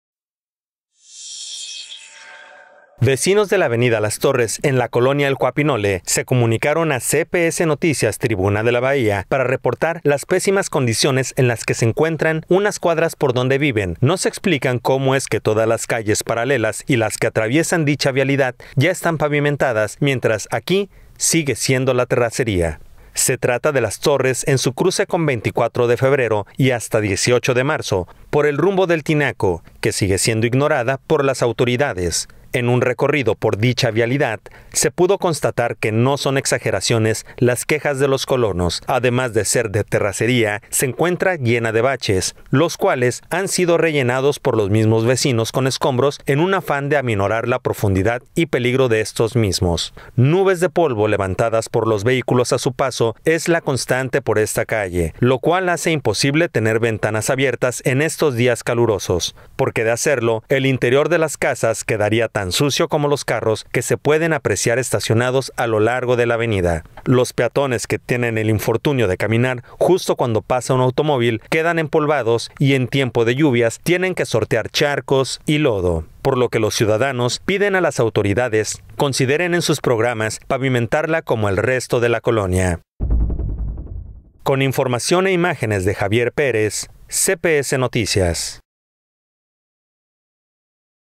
Tramo de avenida Las Torres olvidado | CPS Noticias Puerto Vallarta
Los vecinos no se explican cómo es que todas las calles paralelas y las que atraviesan dicha vialidad ya están pavimentadas mientras aquí sigue la terracería.